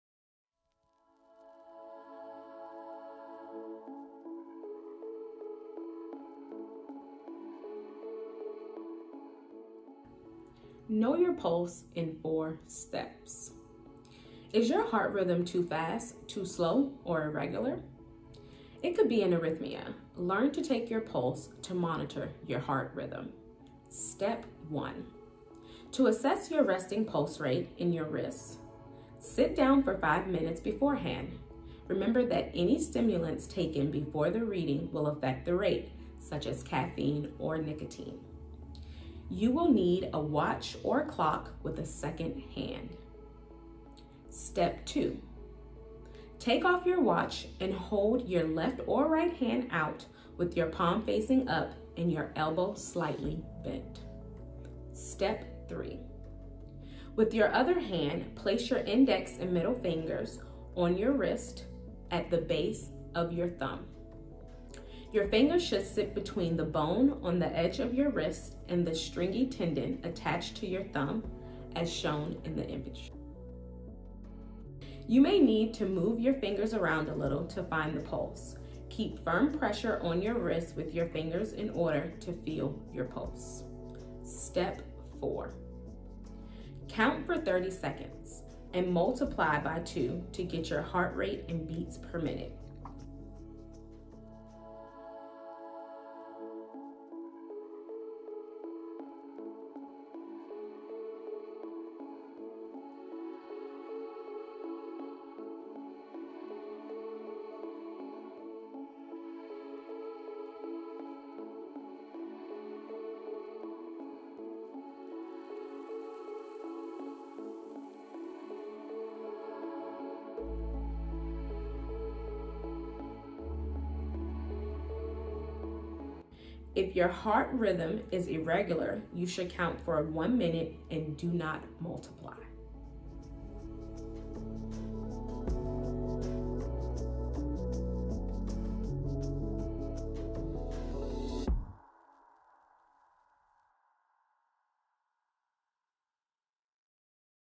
Know Your Pulse
Know Your Pulse to Know Your Heart Rhythm - It Could Save Your Life

Our Know Your Pulse (KYP) program promotes the need for us to be aware of our pulse and for the need to include routine manual pulse checks.